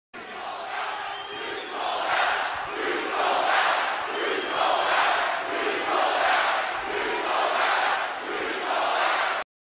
YOU SOLD OUT! WWE Chants (SOUND EFFECT)
Crowd sound effect for wrestling stopmotion animation.